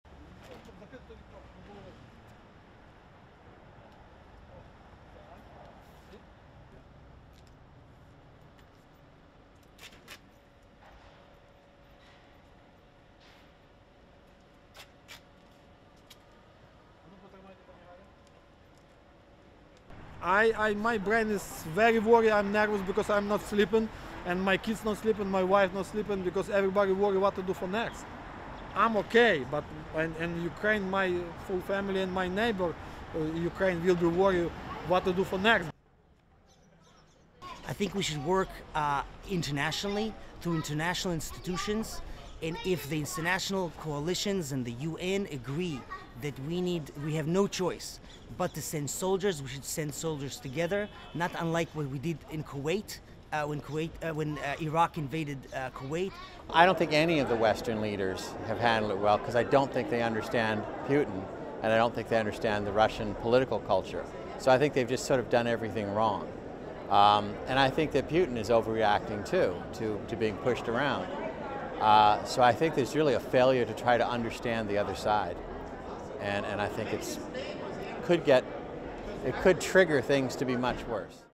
American views on Ukraine invasion vary widely
Americans have a wide range of opinions on Russia's invasion of Ukraine.

A Ukrainian- American living in New York City, Sergiy Bondin, said he and his family are not sleeping at night due to worry.

At the Conservative Political Action Conference meeting in Orlando, Florida, attendee John Mosley said Russian President Vladimir Putin is overacting, but says western leaders have not handled the situation well, either.